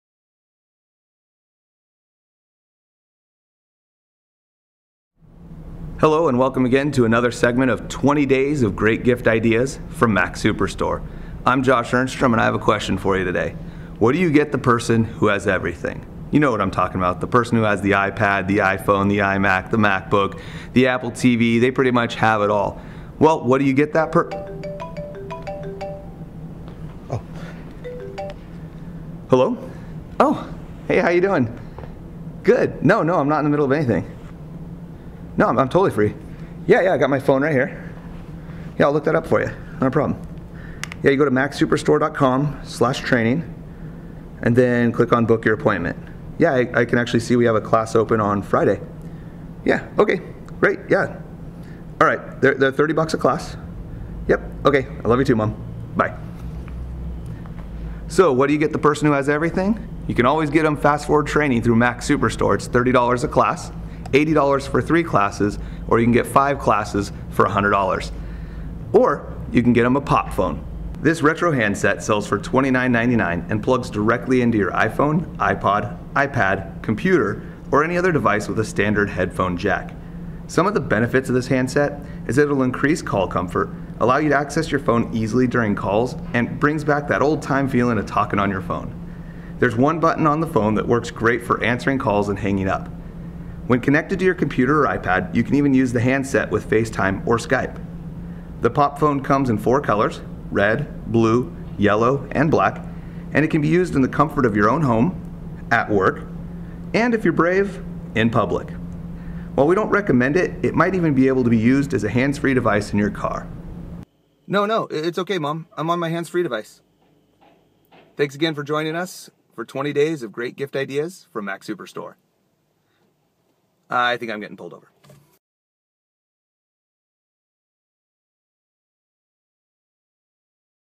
Day 5 - Pop Phone - 20 Days of Great Gift Ideas
This is Day 5 of 20 days of Great gift ideas from MacSuperstore. The Pop Phone is a retro handset that works with your iPhone, iPad, iPod and computer.  This is perfect for the person who has everything.